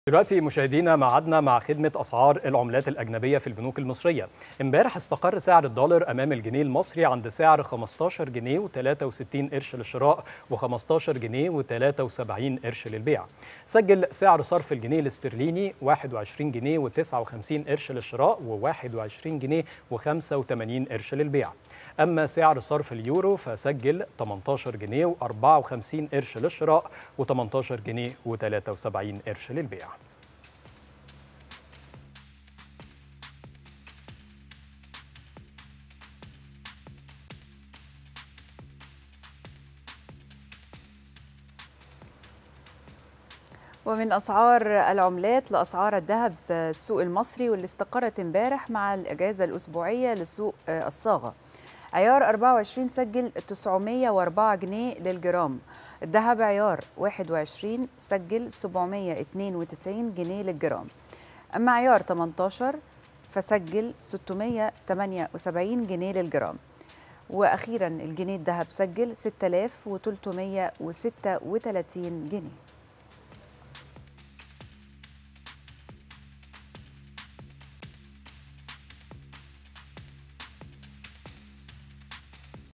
هذا الصباح| استقرار سعر الدولار أمام الجنيه المصري في البنوك.. وأسعار الذهب اليوم
6 سبتمبر 2021